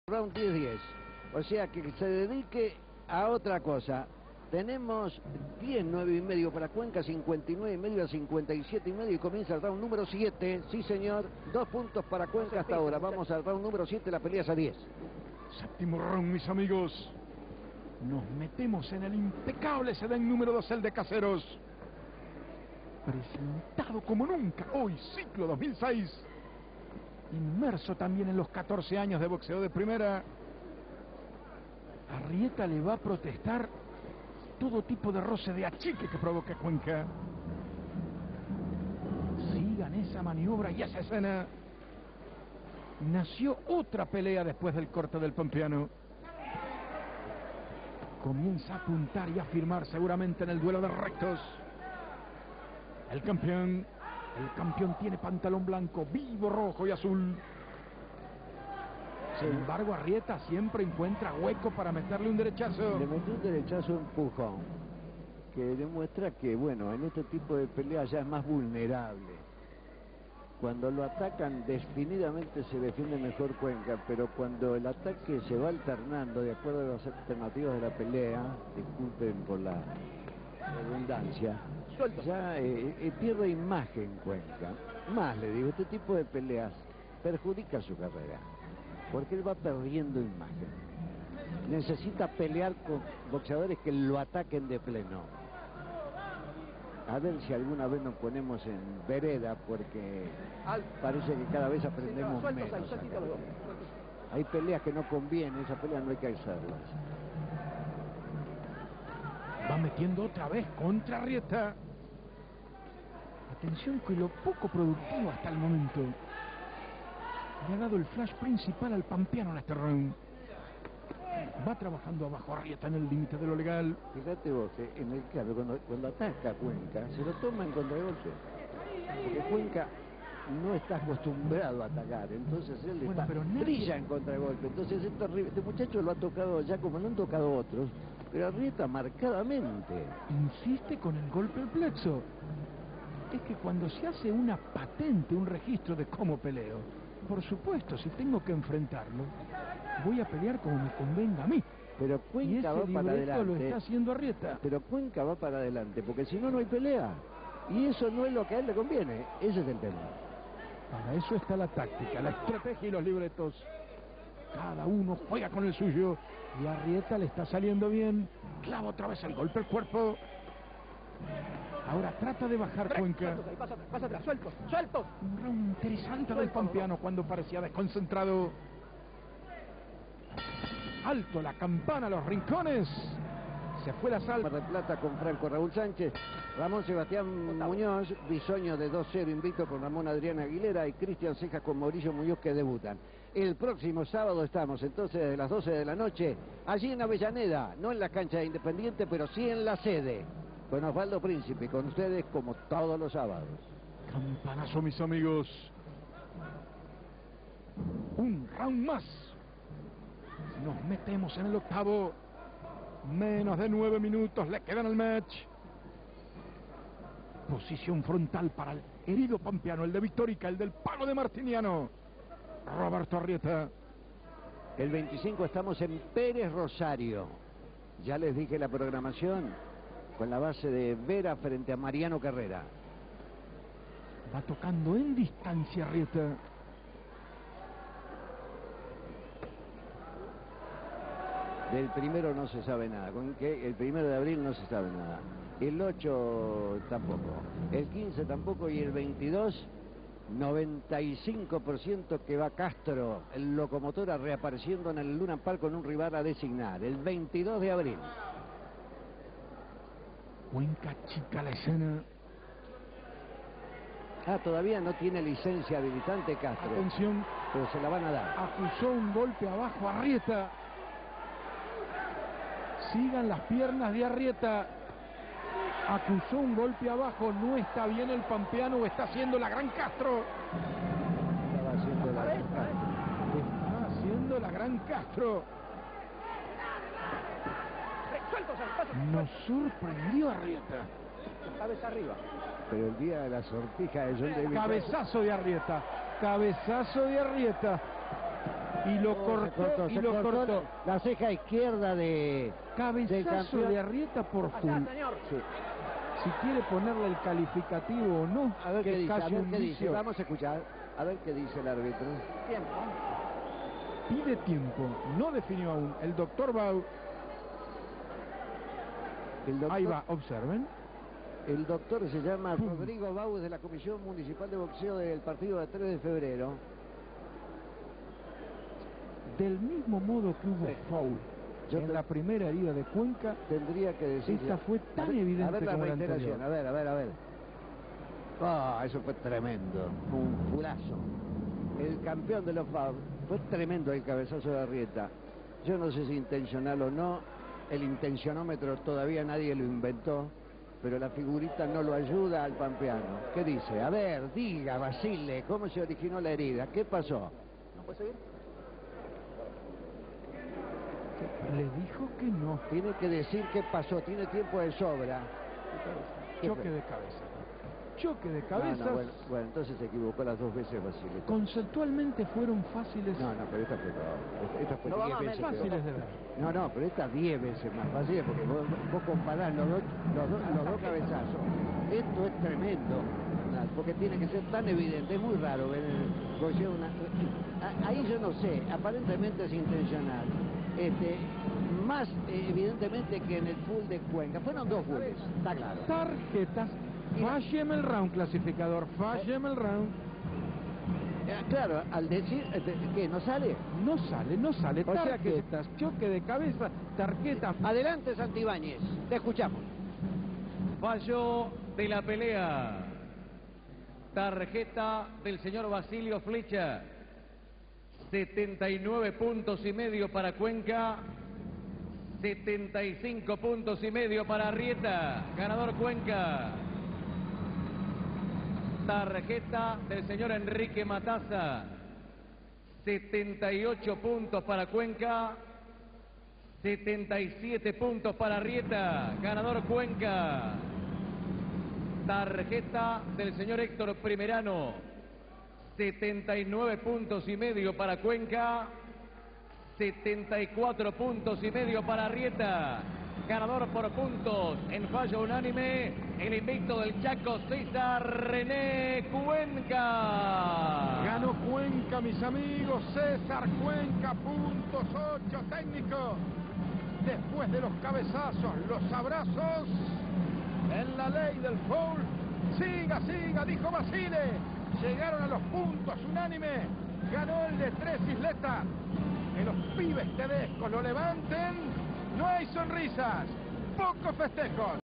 César René "El Distinto" Cuenca vs. Roberto David "Yarará" Arrieta 3ª parte
Pelea realizada el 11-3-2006 en el Ce.De.M. Nº 2, Caseros, Provincia de Buenos Aires